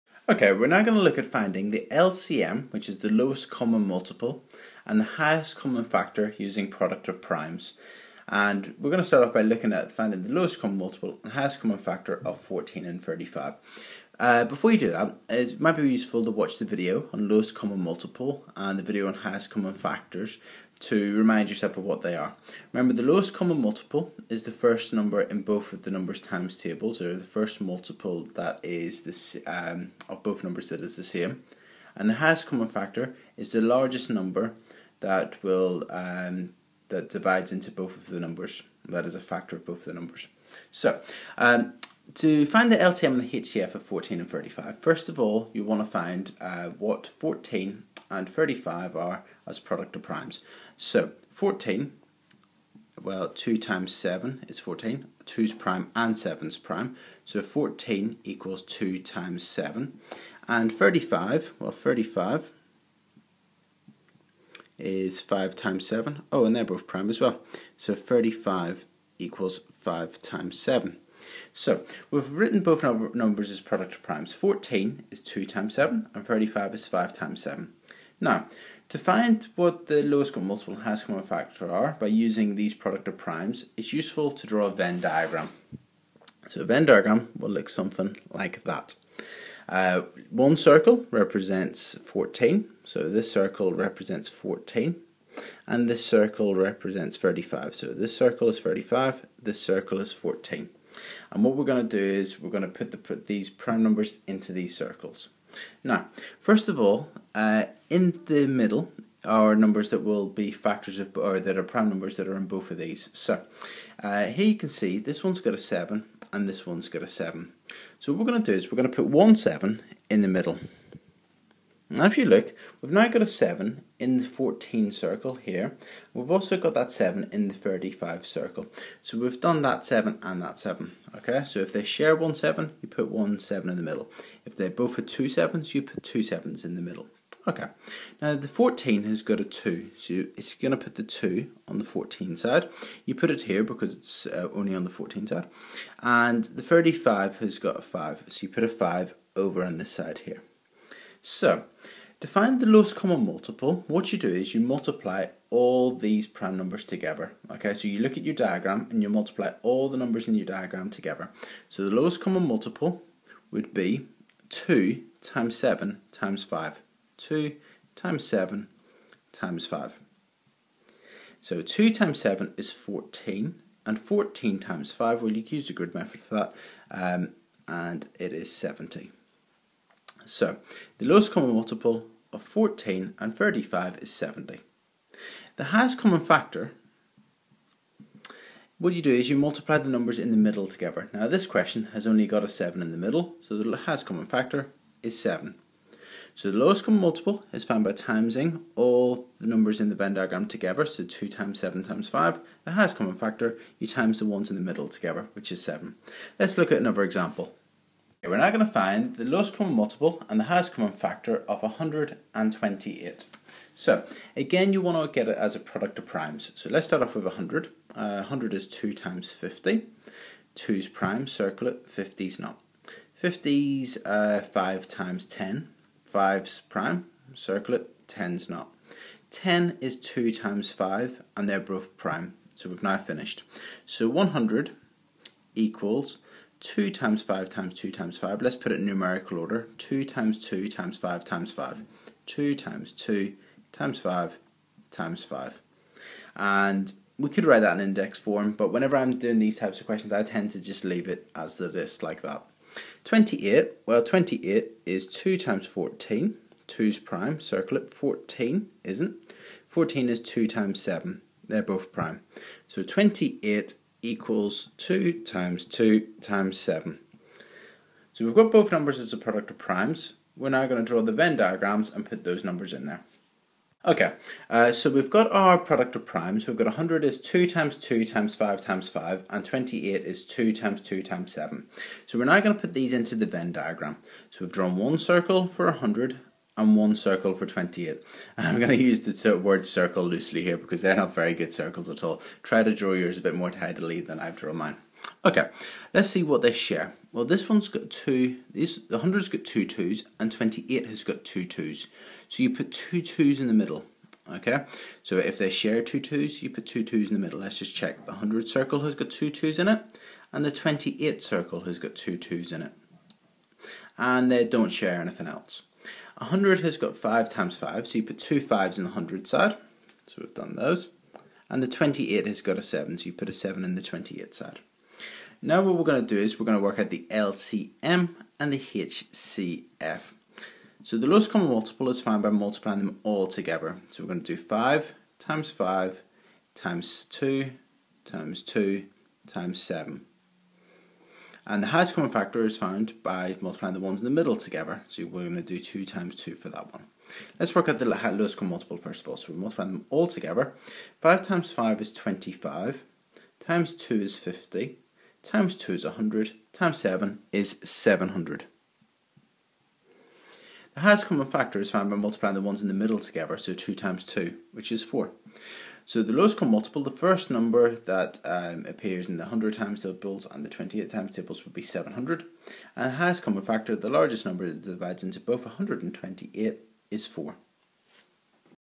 LCM HCF using Product of Primes - Corbettmaths
This video explains how to find the LCM or HCF using product of primes